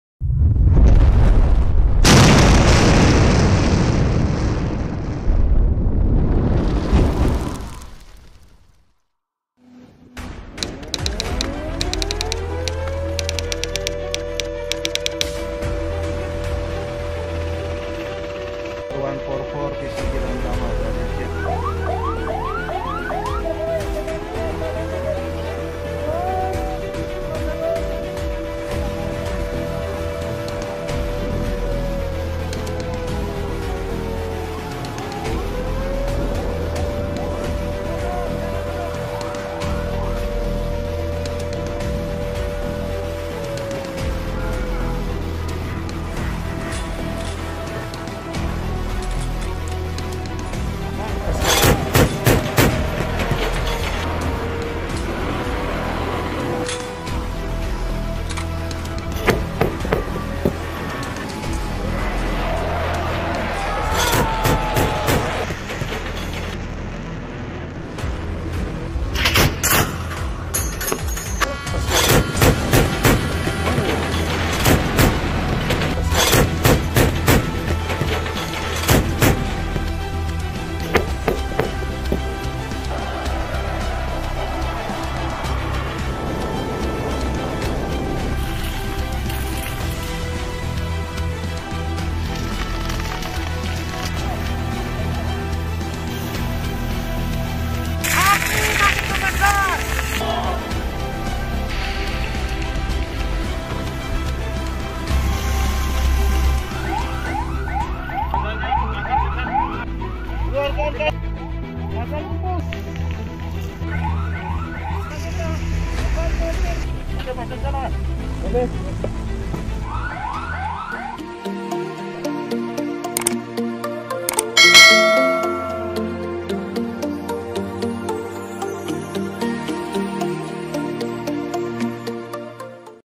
Tembakan Senjata Anti Serangan Udara Lantamal VI Berhasil Jatuhkan Dua Pesawat Tempur Musuh
“Ini adalah salah satu bagian dari latihan drill  Gladi tugas tempur atau disingkat Glagaspur  P-1/P-2 dan L-1/L-2 untuk menguji kemampuan persenjataan anti pesawat udara Lantamal VI dalam mempertahankan wilayah dari serangan udara”, kata Asisten Operasi (Asops) Danlantamal VI Kolonel Laut (P) Ardhi Sunaryo S.T, M.M